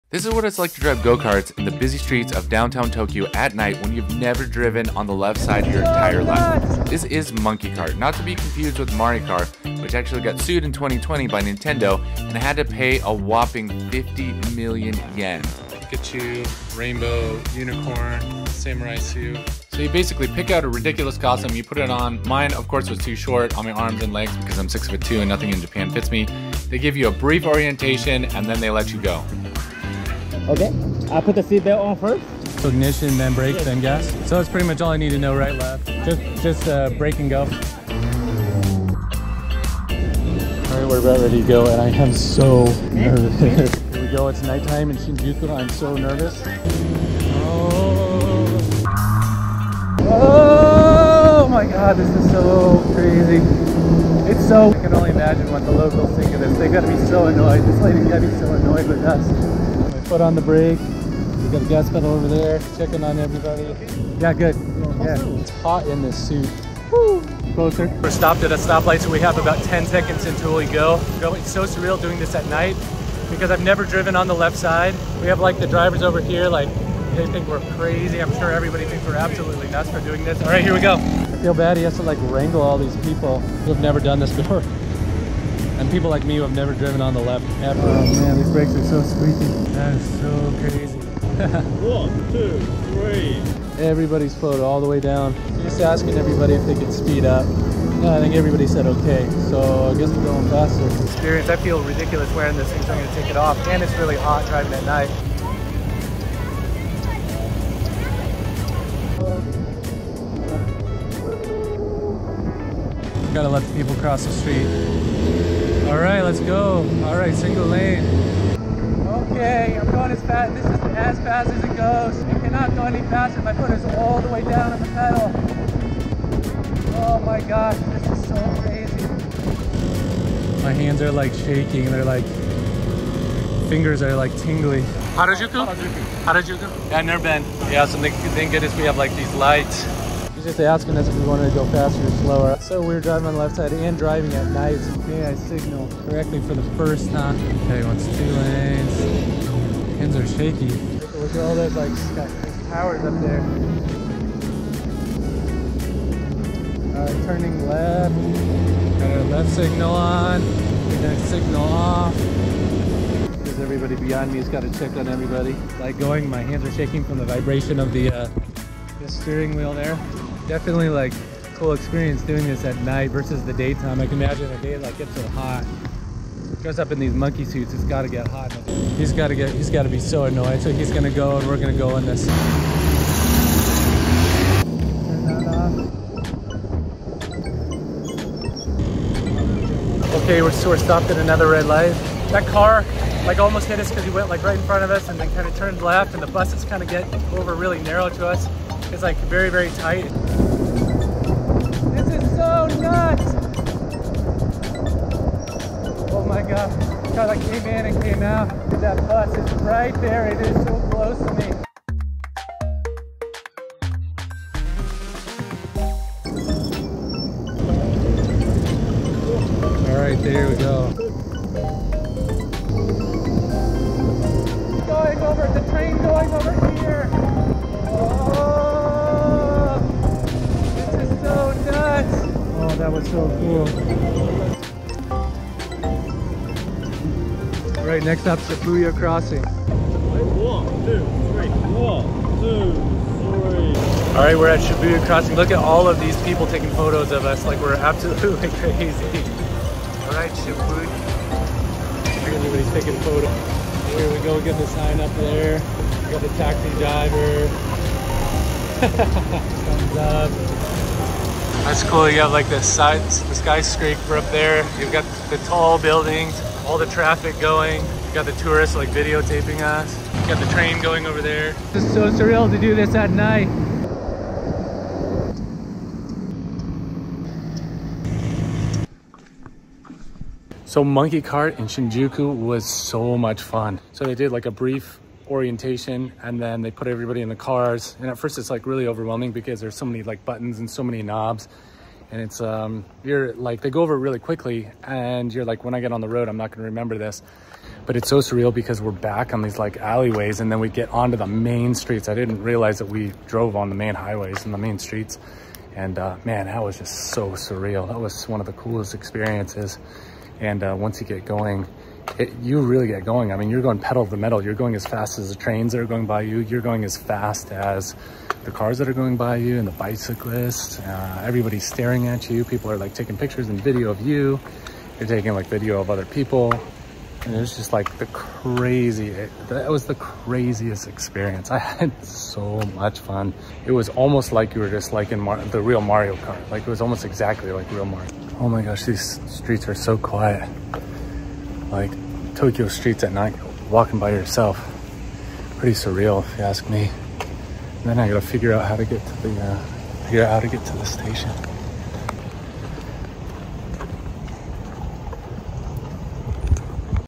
Go karting Tokyo at night
driving in tokyo at night on the left side is scary!